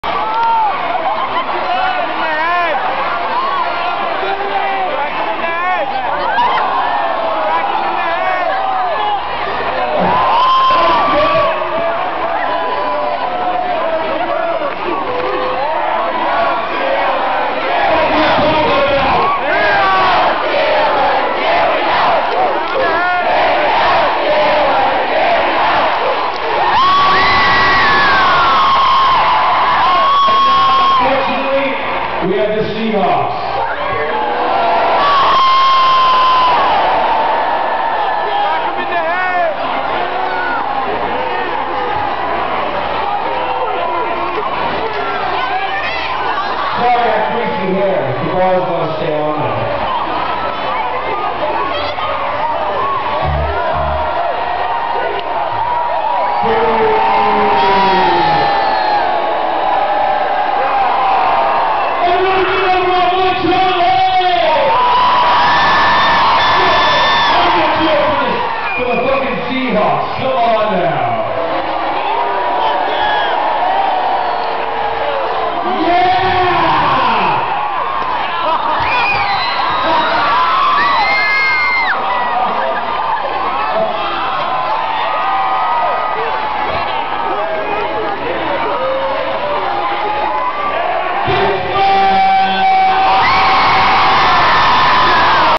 Fall Out Boy's Dirty
Intermission of the Black Clouds and Underdogs show involving Dirty, Charlie, a baseball bat, a wiffle ball, the Steelers and the Seahawks. Yeah.. it was interesting